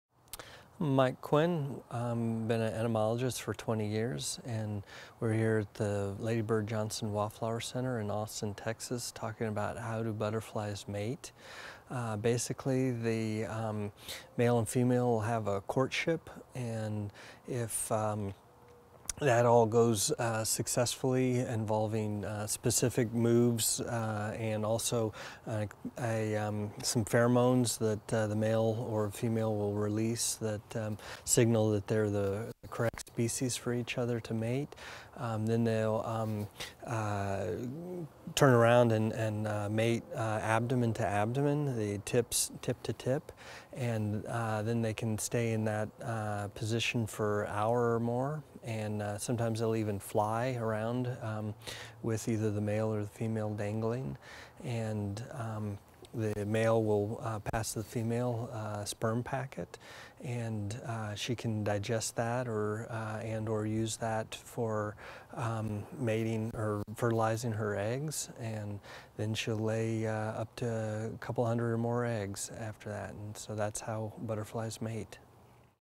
Insect Information : How Do Butterflies Mate?
When butterflies mate, the male or female will perform courtship movements and release pheromones, the butterflies will turn abdomen to abdomen and then mate for an hour or more. The male butterfly will pass the female a sperm packet that is used for fertilizing eggs. Discover more about how butterflies mate and lay several hundred eggs with information from an entomologist and insect specialist in this free video on butterflies.

Expert: Mike Quinn
Bio: Mike Quinn has a Master of Science degree in entomology from Texas A&M University.